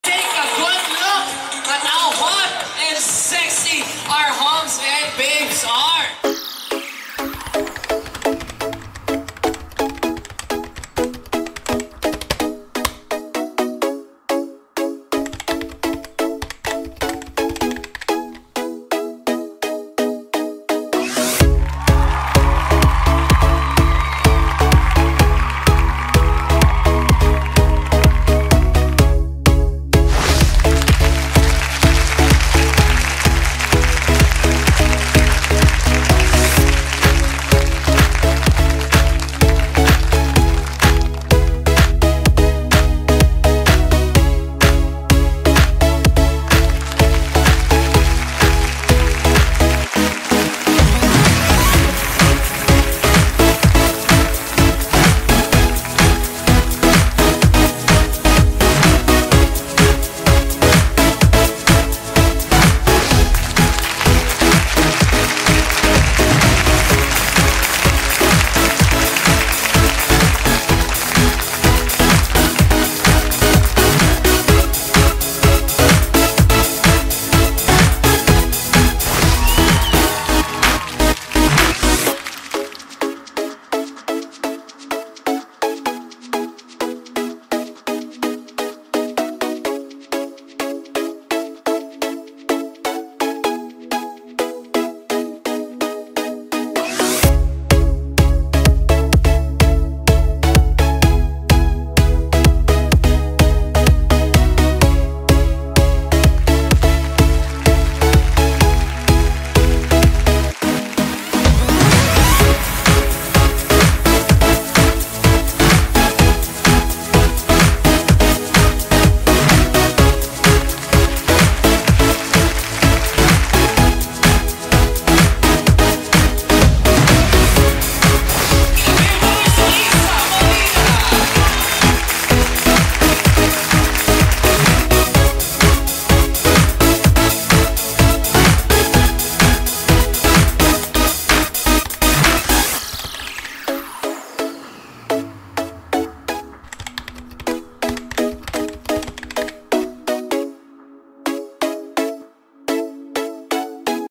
Ysa Molina - Summer Bodies Quest 2024 (10th Elimination)
Ysa Molina's performance in the 10th elimination round of 'Summer Bodies Quest Year 15' held at Isetann Recto on May 18, 2024.

Ysa has progressed to the grand finals.